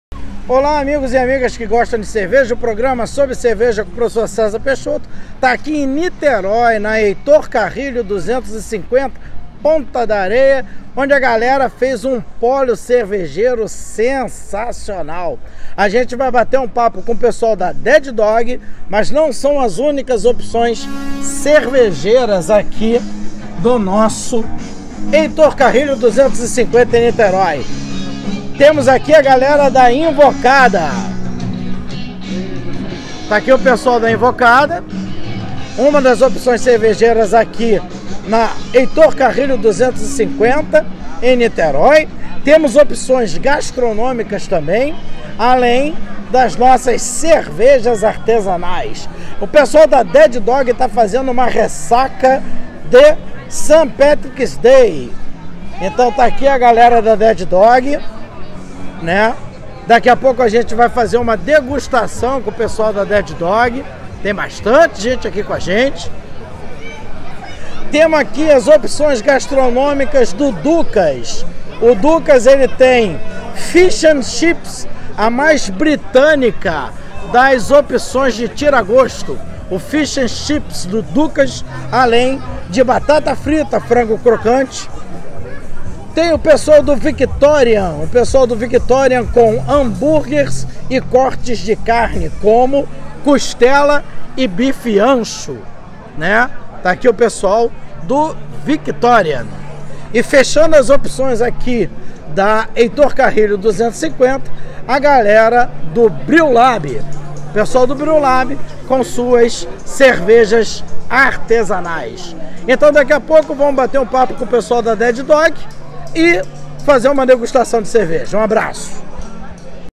Sobre Cerveja no Pólo Cervejeiro da Ponta da Areia em Niterói
Sobre Cerveja com Professor Cesar Peixoto no pólo cervejeiro da Ponta da Areia, em Niterói, na Rua Professor Heitor Carrilho, 250